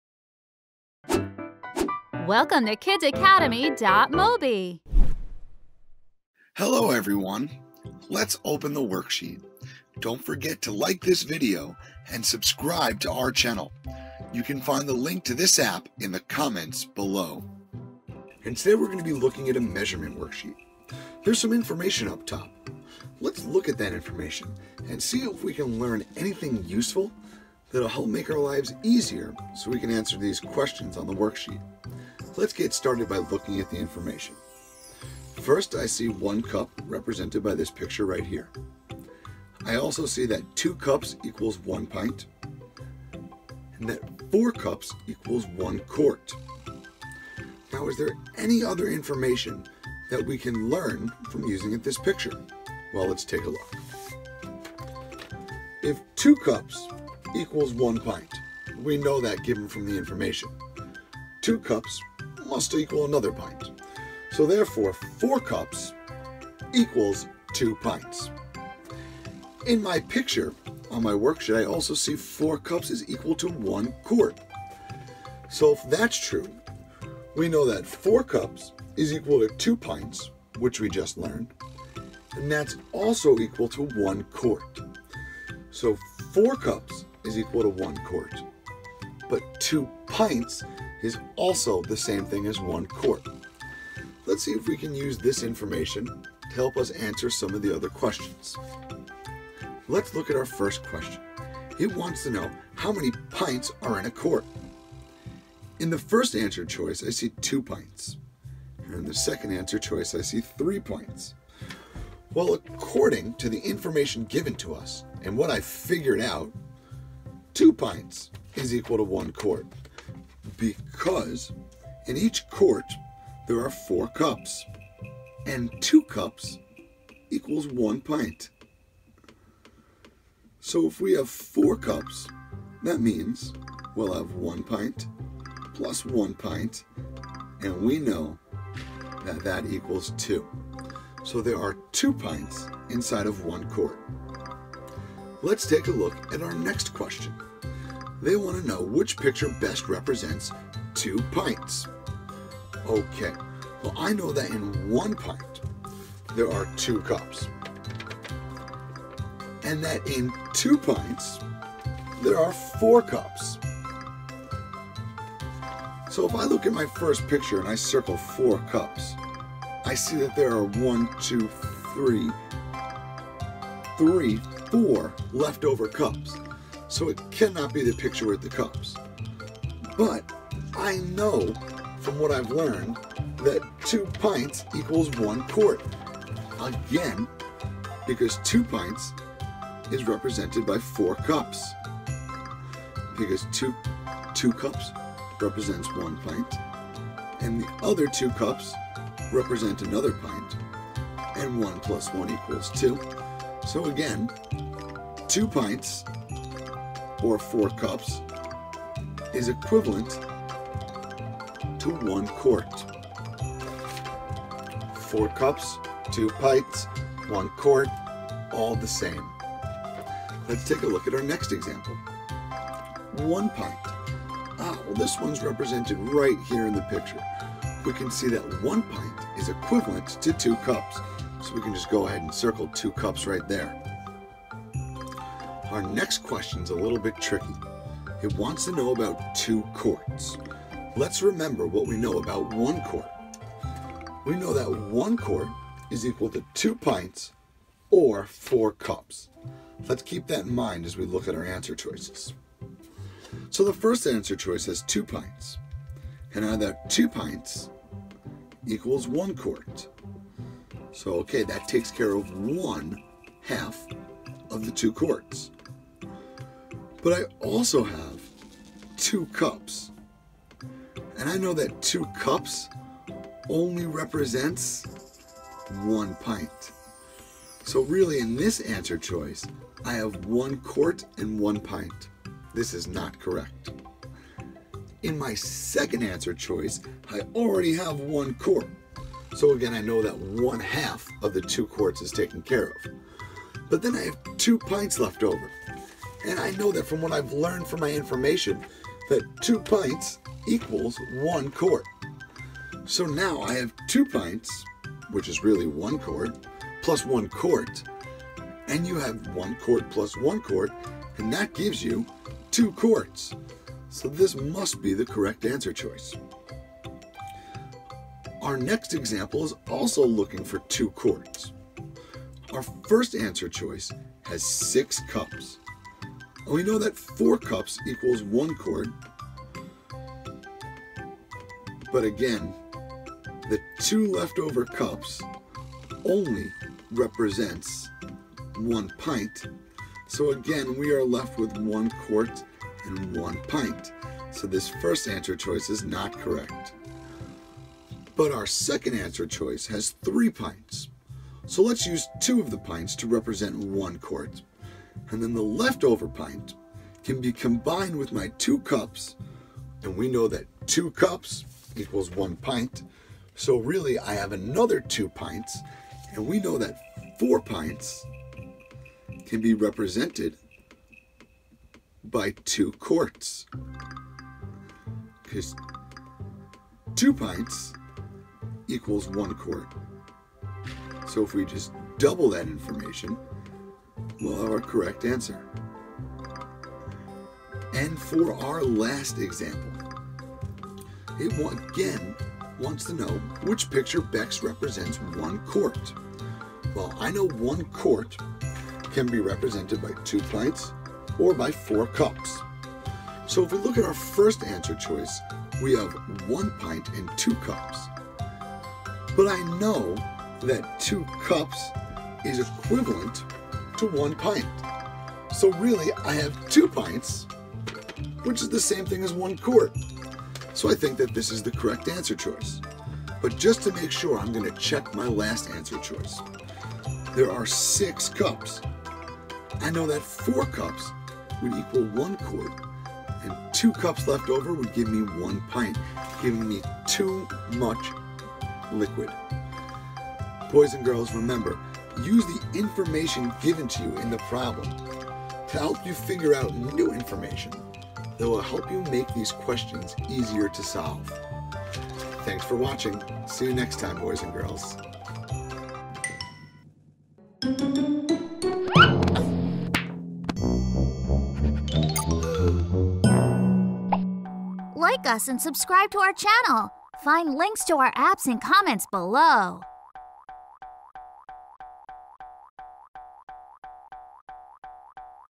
Measurement for Kids - Capacity: Cups, Pints and Quarts - part 2 | Kids Academy
Teacher Mike is back again to continue your child’s learning of cups, pints, and quarts in part 2 of our measurement video tutorials! This time, your child will expand his or her learning to include even trickier problems, including gallons, in this engaging installment.

Like part 1, this video will improve your child’s:
• Ability to measure liquids in cups, pints, quarts, and gallons
• Understanding and memorization of capacity

Learning measurement is a long process since it includes so many types and units of measurement. This video tutorial makes learning to measure liquids easy with an experienced teacher leading the way, using relevant pictures and engaging narration to increase and solidify learning!